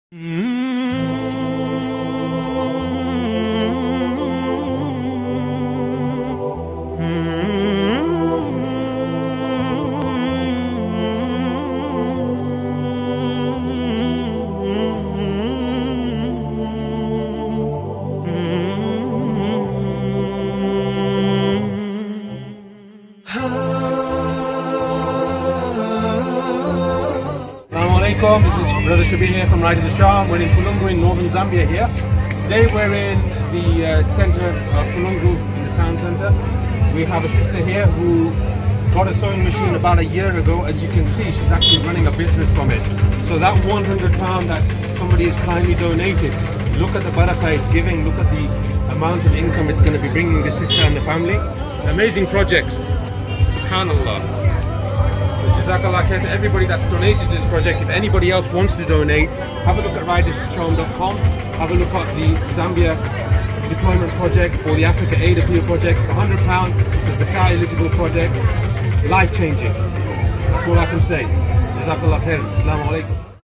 sewing machine project in zambia
This sowing machine project gives the poor people a chance to earn a halal income